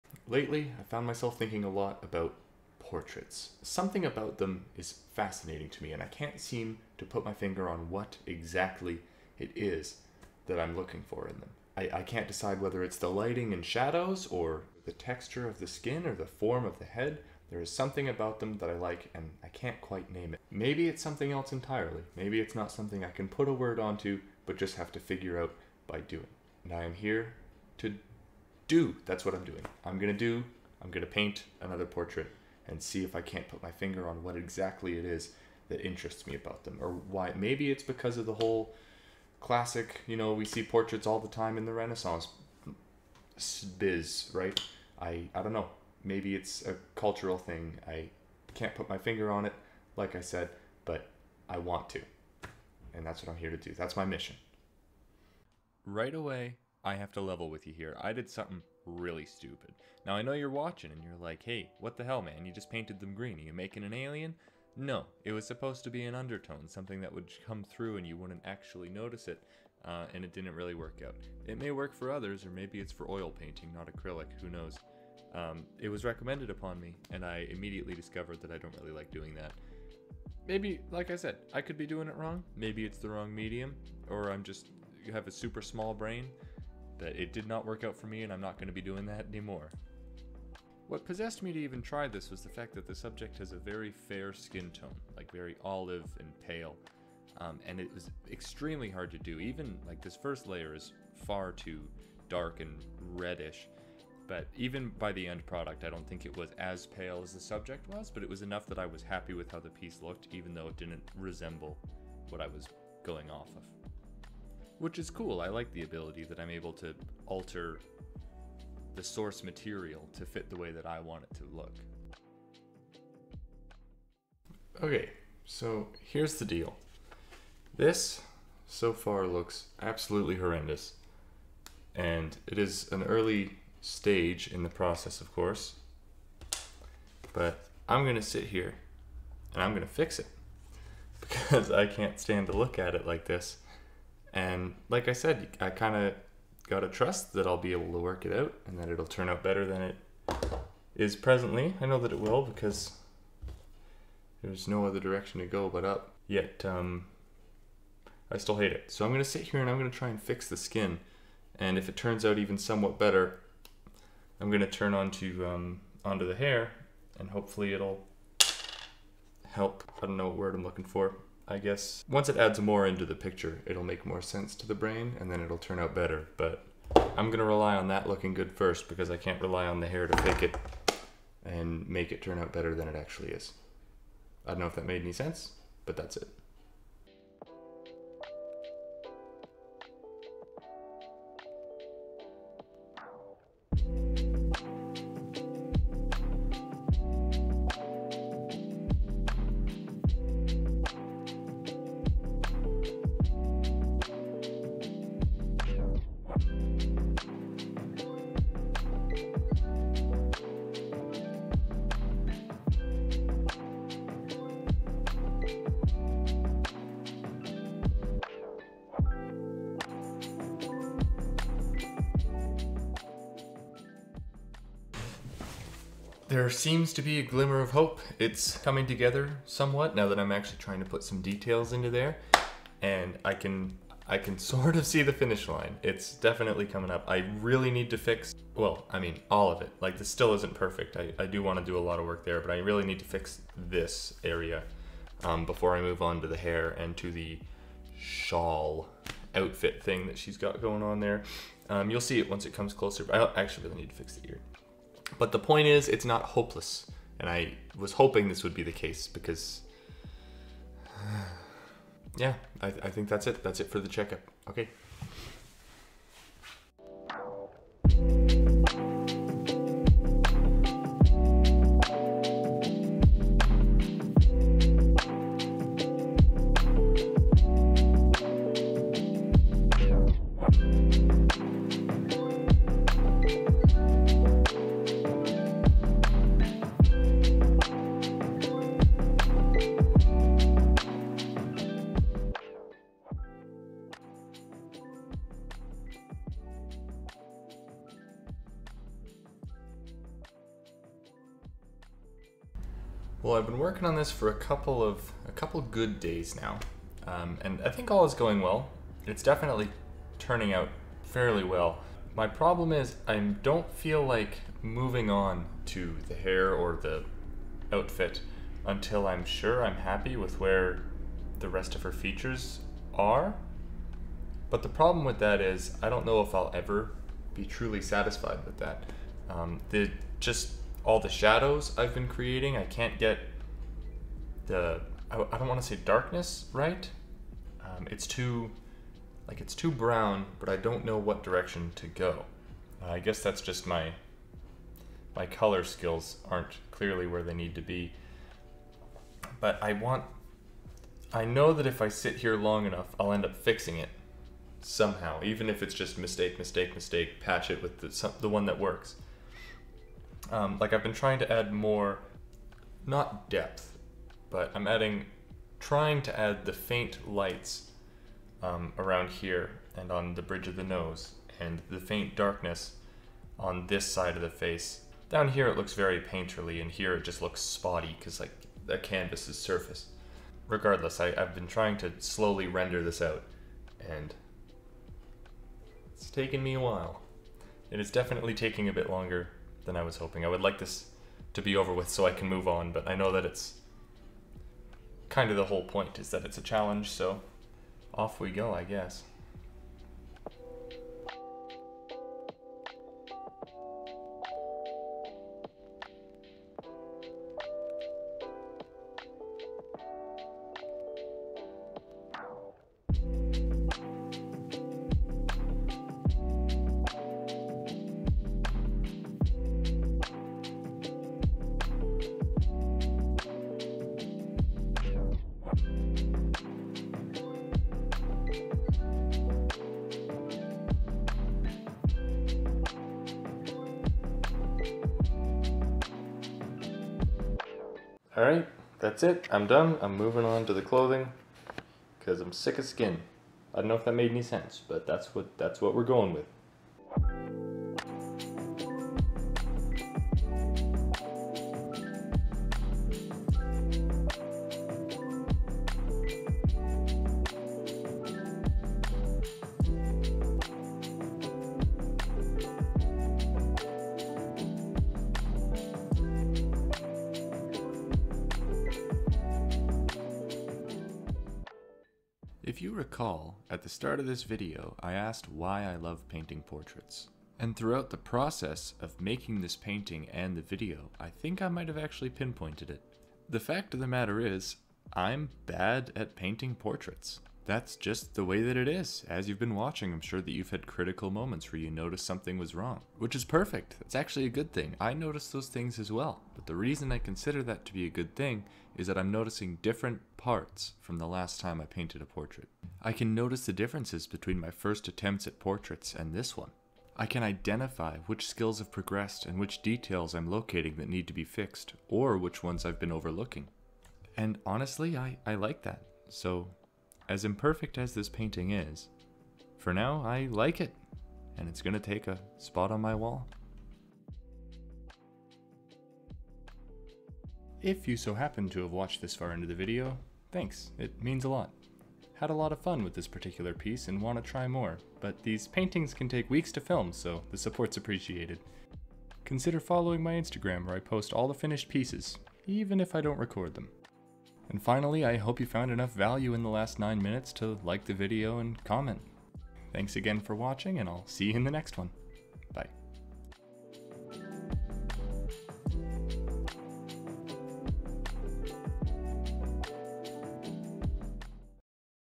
Why are portraits so satisfying? | Entire painting process
This video is the process of painting a portrait, from beginning, middle to end. Lot's of new techniques used and a whole new video format :)

12x12 acrylic on canvas

Chapters:
0:00 Opening
0:55 Painting [1]
2:04 Meeting
3:06 Painting [2]
3:44 Checkup
4:25 Painting [3]
5:00 Progress check
6:41 Painting [4]
7:51 Final thoughts
8:48 Varnishing / Sendoff

Tunes!:
Music from Uppbeat (free for Creators!):